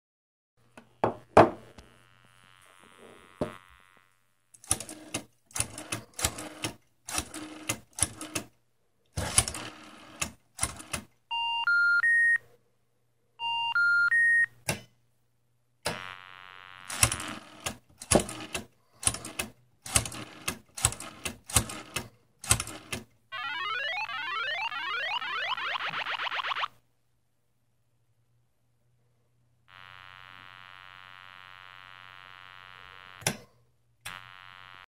Rotary phone lock puzzle 2
Testing the escape room rotary phone lock.
When the correct number is dialed 5v is sent to a relay for 3 seconds.
Still some work to do on sounds and the number array management...